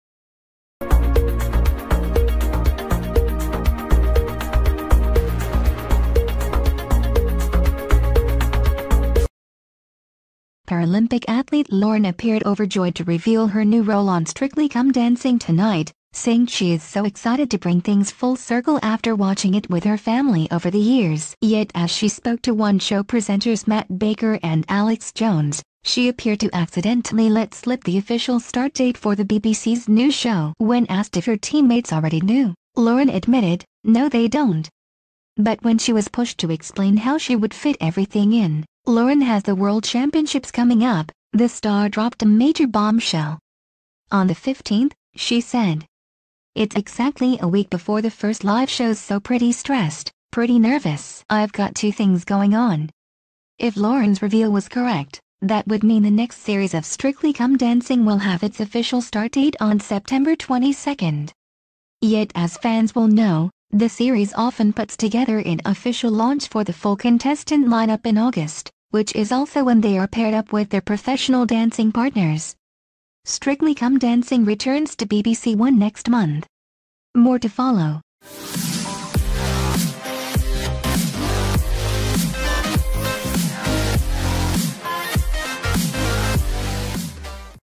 Strictly Come Dancing 2018: Lauren Steadman accidentally REVEALS BBC show's start date?
Paralympic athlete Lauren appeared overjoyed to reveal her new role on Strictly Come Dancing tonight, saying she is so excited to bring things full circle after watching it with her family over the ye